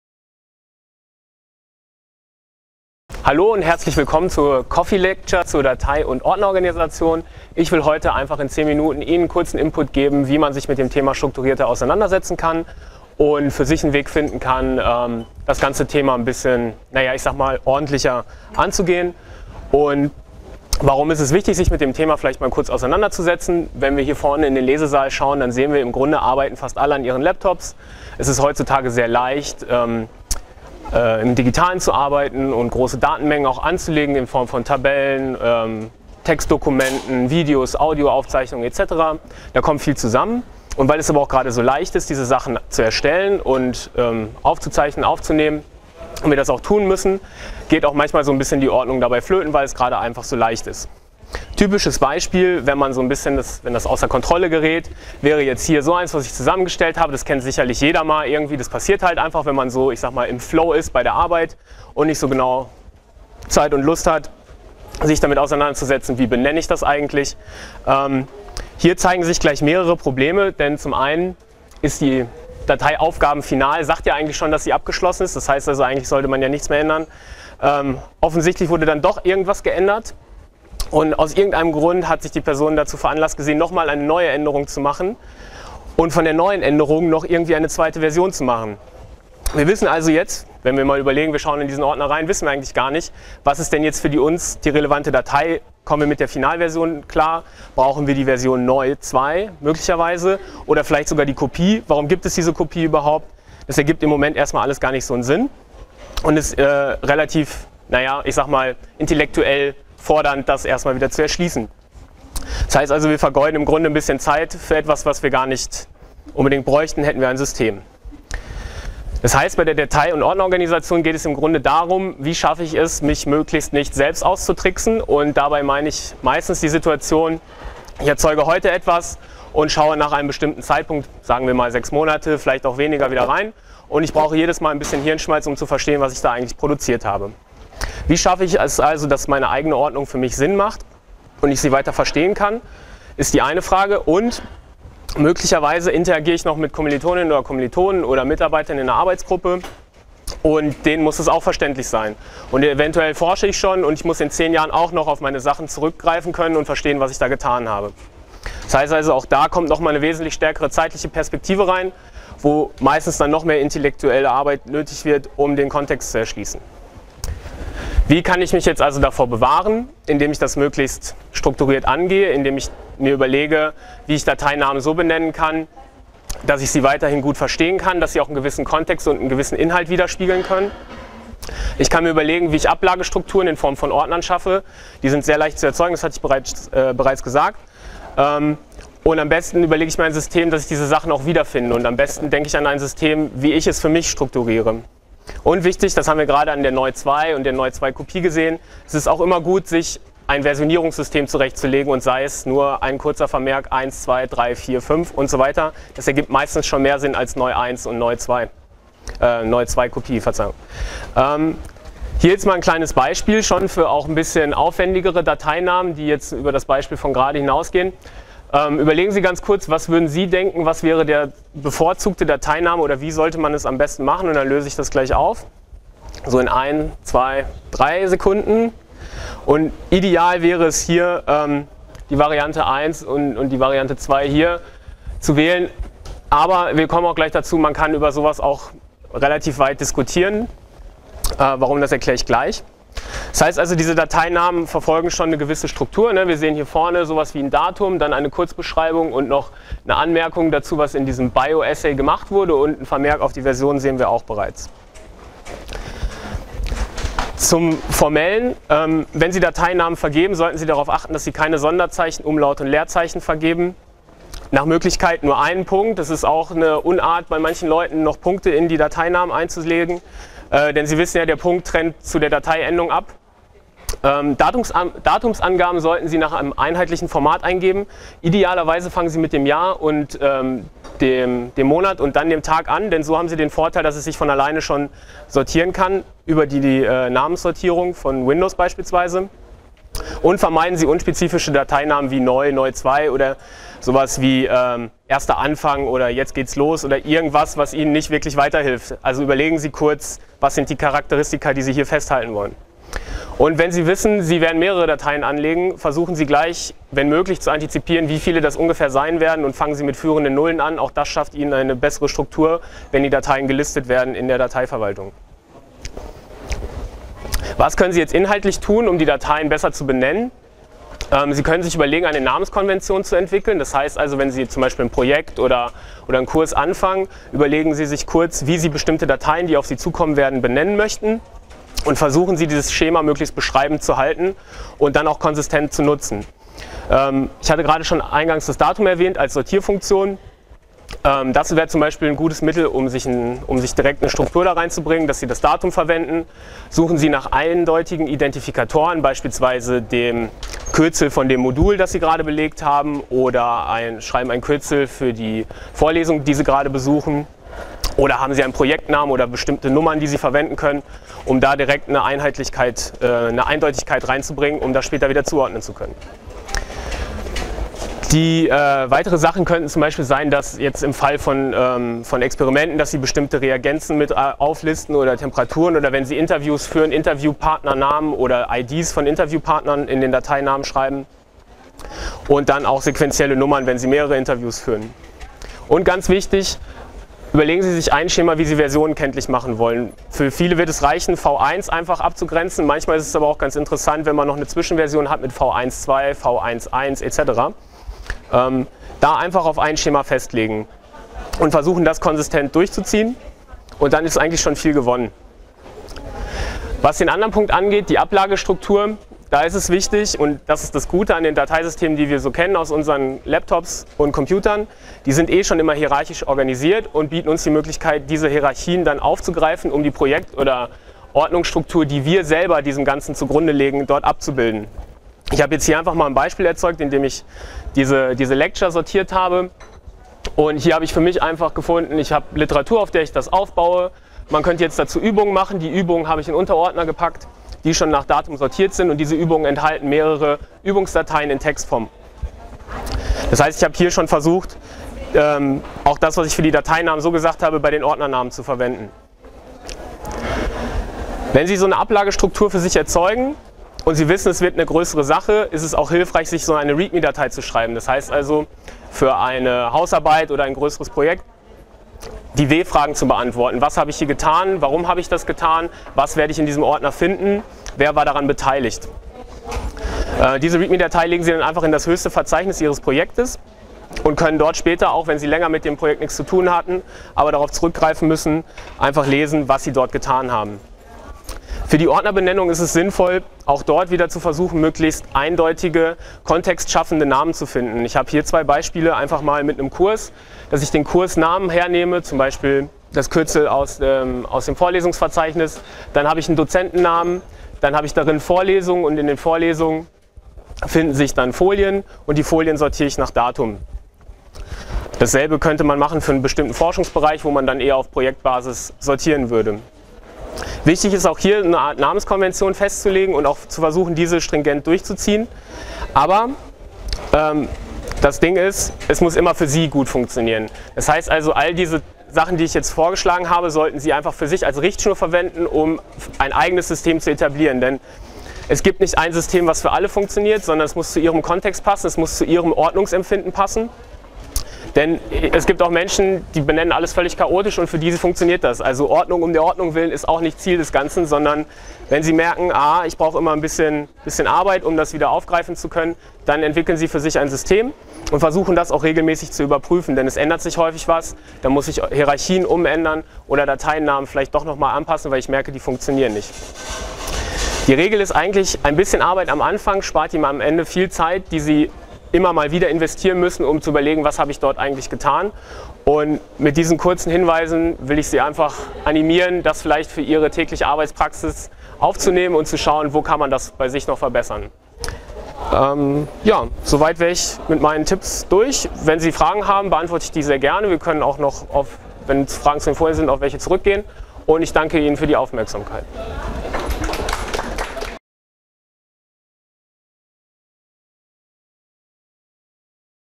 Datei- und Ordnerorganisation
Dieses Video ist ein Mitschnitt der Coffee-Lecture "Datei- und Ordnerorganisation" vom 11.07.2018 von Christian Krippes (Forschungsdatenreferent).

▶️ Inhalt:
00:00 Intro
00:03 Einleitung Datei- und Ordnerorganisation
00:53 "Warum ist das wichtig"?
02:47 "Wie geht das"?
03:29 Dateinamen
06:46 Ablagestruktur
08:38 Fazit
09:38 Ende